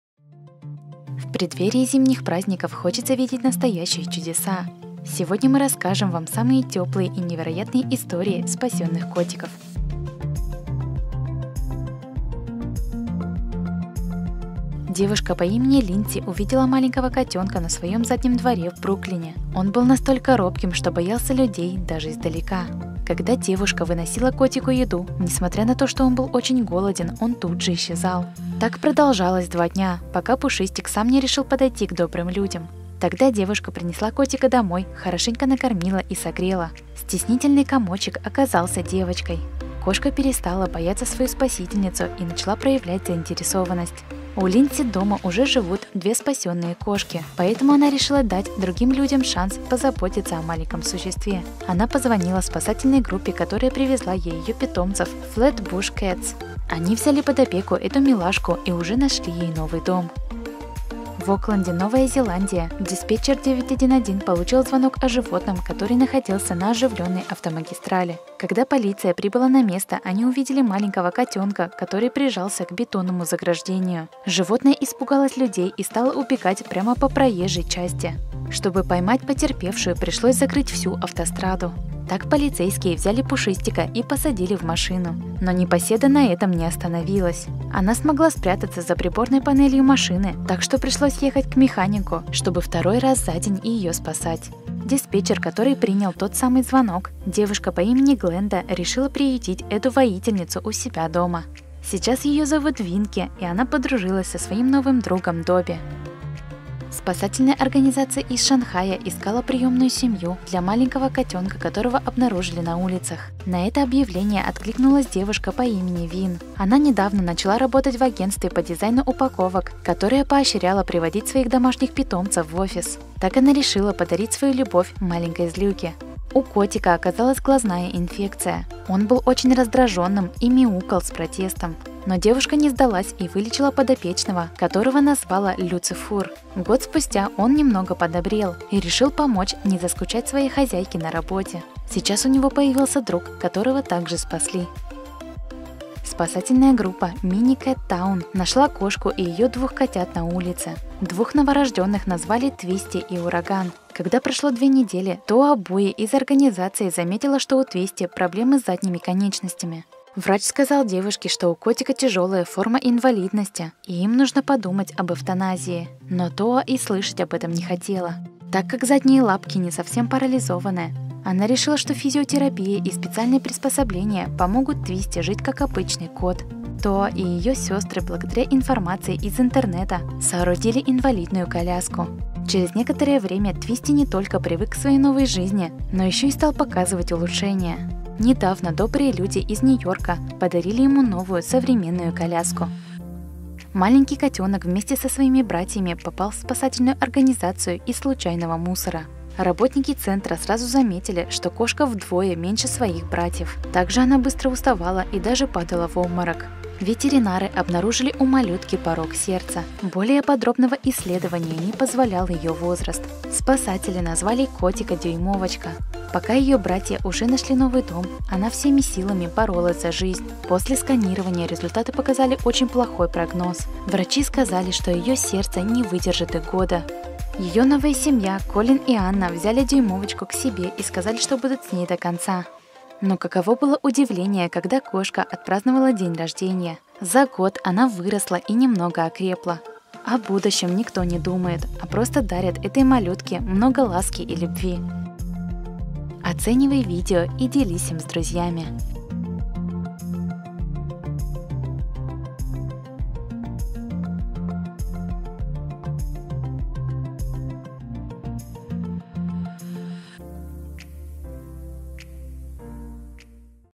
ИСТОРИИ СПАСЕННЫХ КОТИКОВ #6
В преддверии зимних праздников хочется видеть настоящие чудеса. Сегодня мы расскажем вам самые теплые и невероятные истории спасенных котиков.